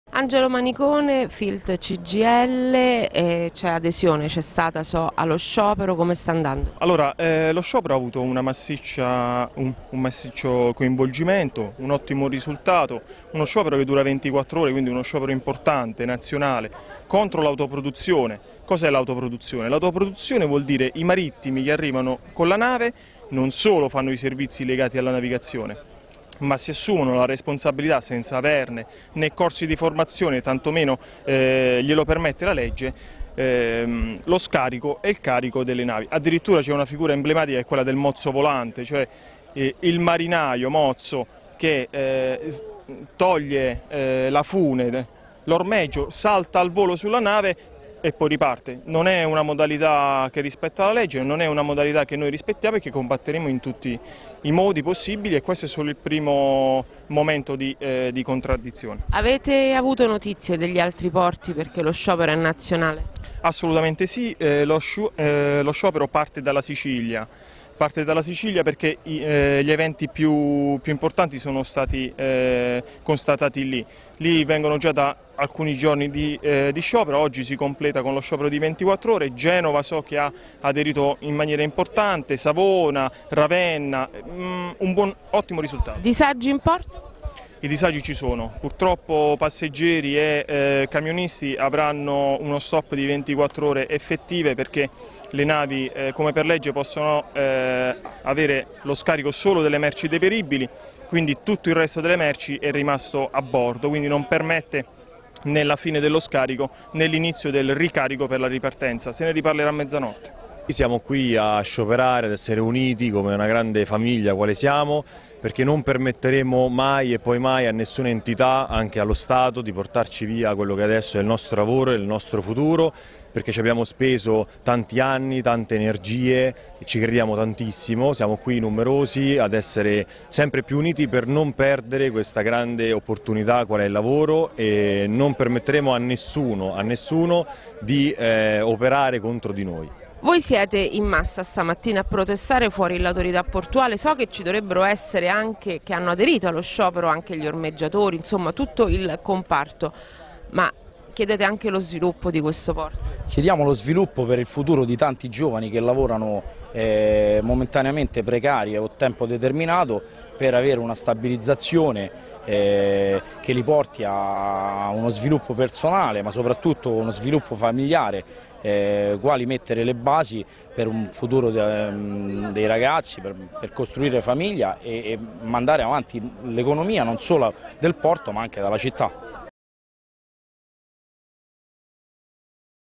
Filt Cgil: ottimo risultato per lo sciopero portuale
Il commento dei portuali sullo sciopero nazionale indetto da Cgil, Cisl e Uil contro l'autoproduzione.